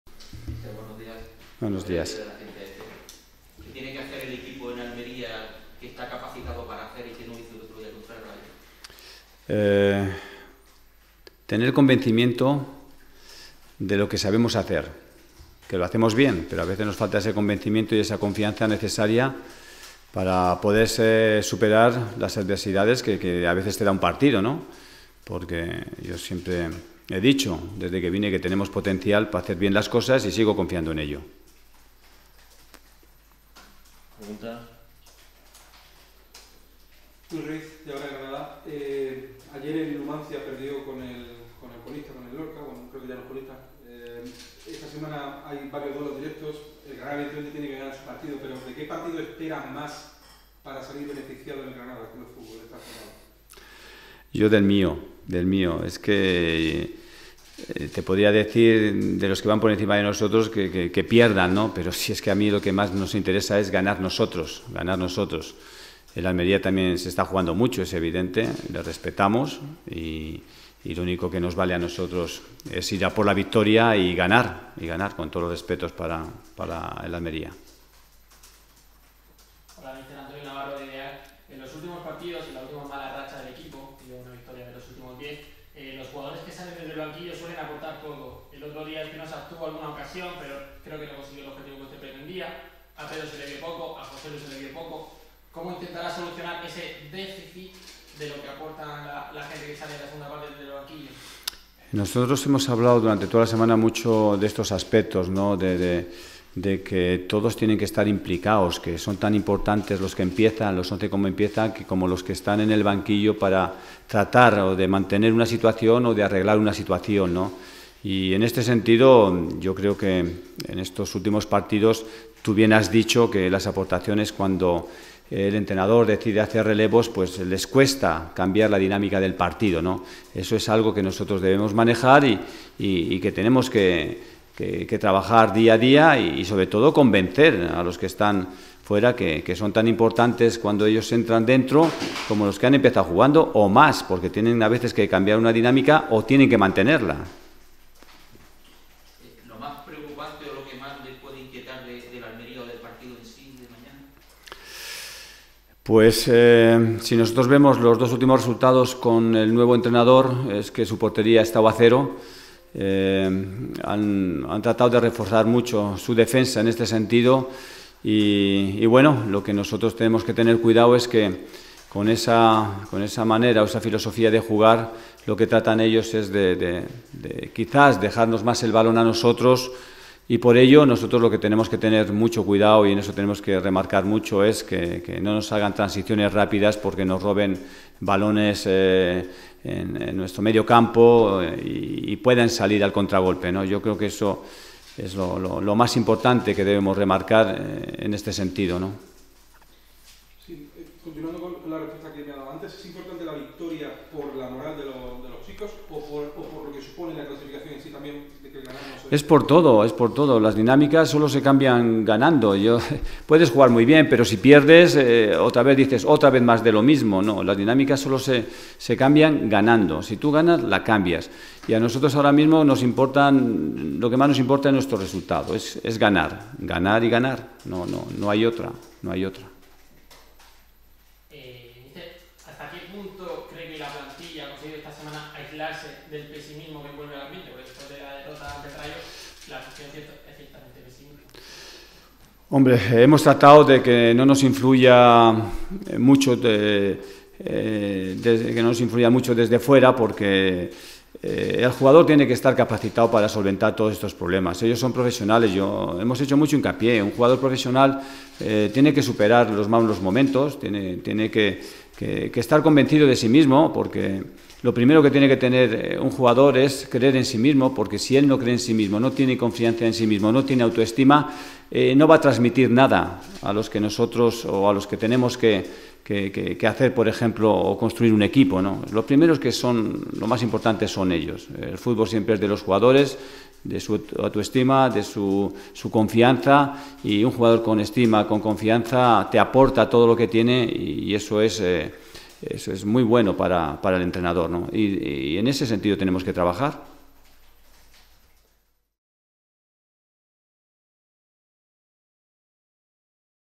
Portugal: "Nos importa el resultado: ganar y ganar"
A Miguel Ángel Portugal, entrenador del Granada CF, sólo le interesa ganar. Es esa victoria en Almería la que puede cambiar la dinámica del equipo de las últimas semanas y avivar las aspiraciones de ocupar un puesto en el 'play off' de ascenso.